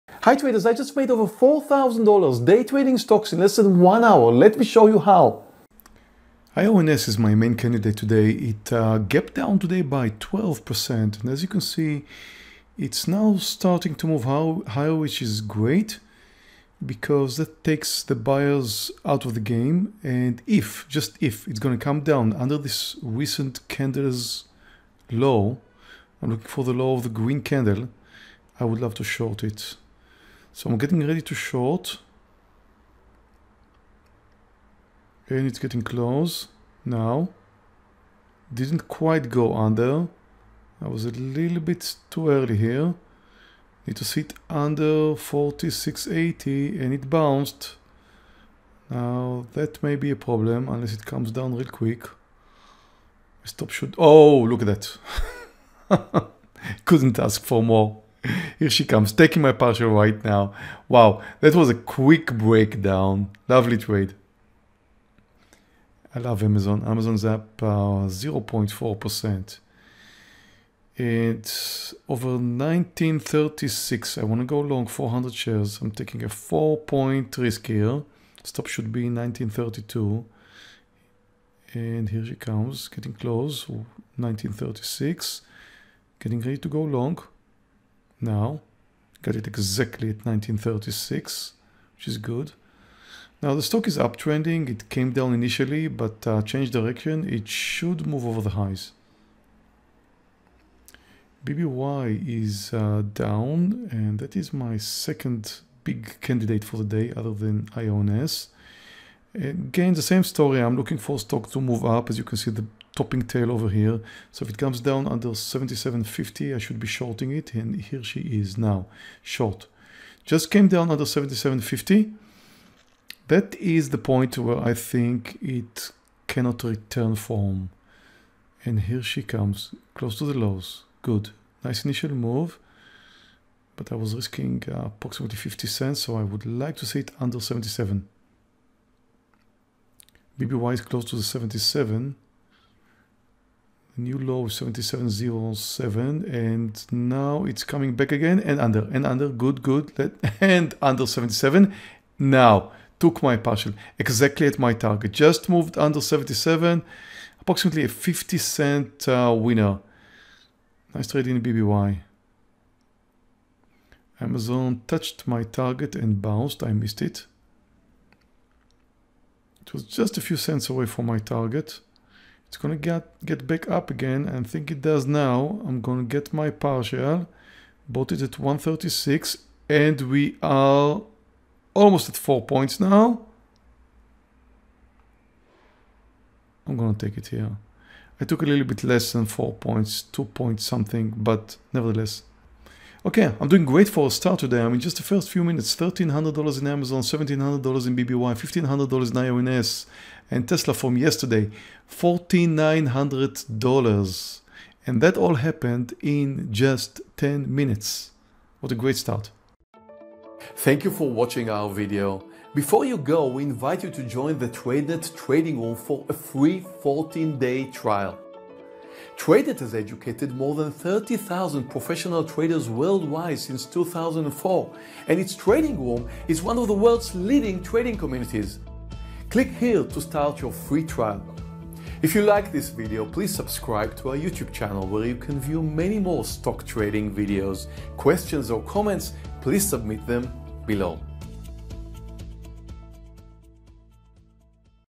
Live Day Trading For $4,900 in 10 Minutes!
Meir Barak day trading live on August 29th. 2018.